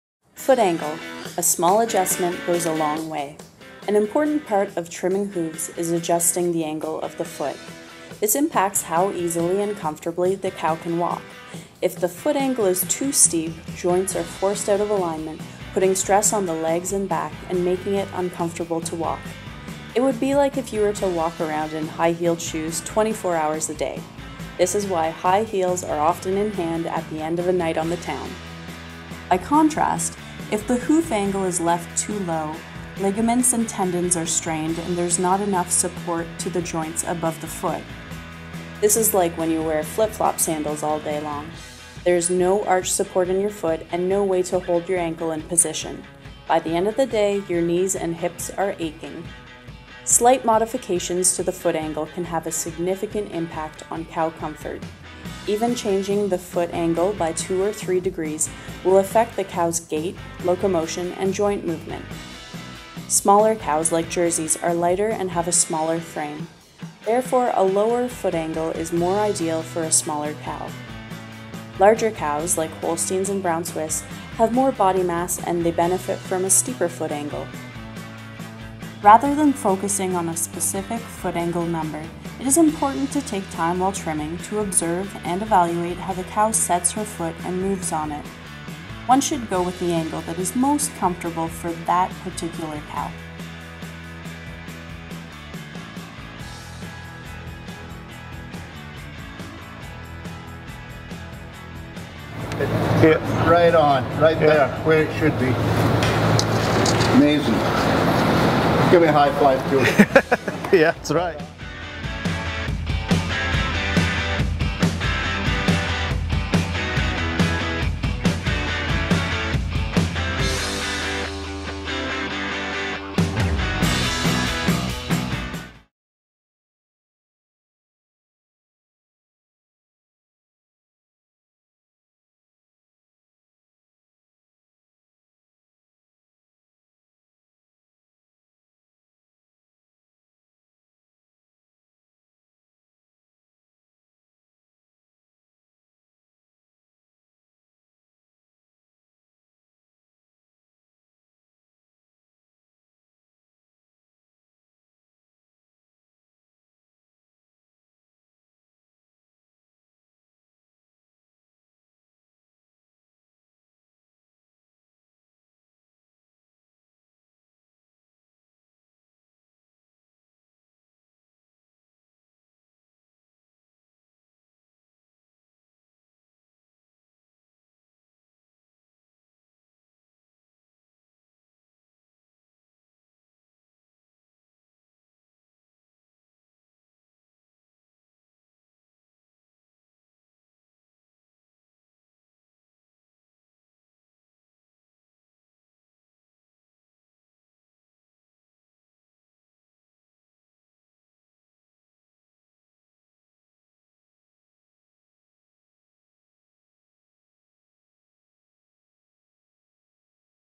Foot Angle: A Small Adjustment Goes a Long Way
Slight modifications to foot angle can have a significant impact on cow comfort. Even changing the angle by 2 or 3 degrees will affect the cow’s gait, locomotion, and joint movement.